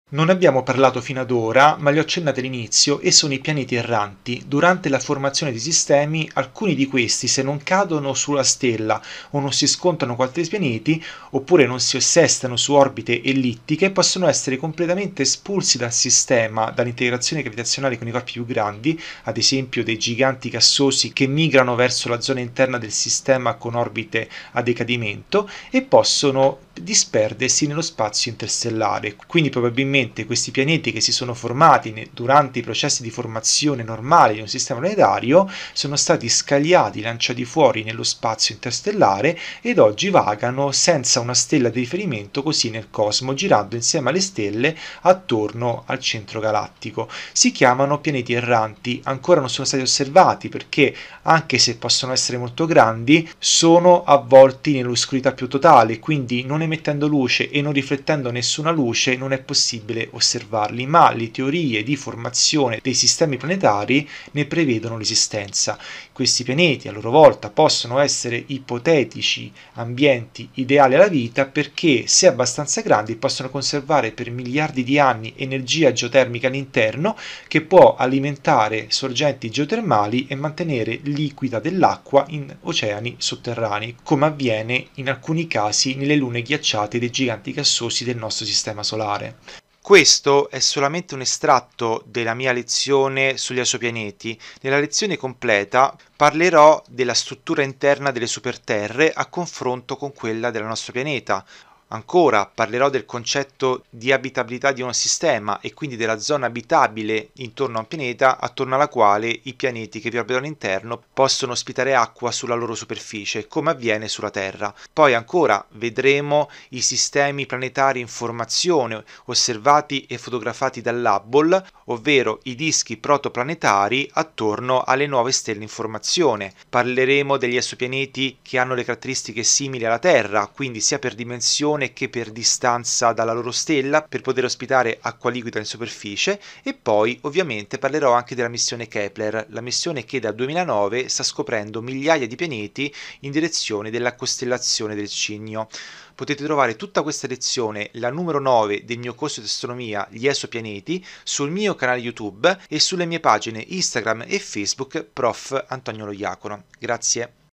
I PIANETI ERRANTI - Orfani di Stelle
Non tutti i pianeti orbitano attorno ad una stella, le simulazioni al computer elaborate dagli scienziati per studiare la formazione dei sistemi planetari, mostrano che non è raro che molti di essi espellano dei pianeti che poi finiscano col vagare orfani nella galassia senza un sistema di appartenenza. QUESTI SONO I FAMISI PIANETI ERRANTI. Possibili forme di vita potrebbero evolversi in oceani sotterranei alimentati da fonti geotermiche.

VISITA LE MIE PAGINE SOCIAL :

Prof. Antonio Loiacono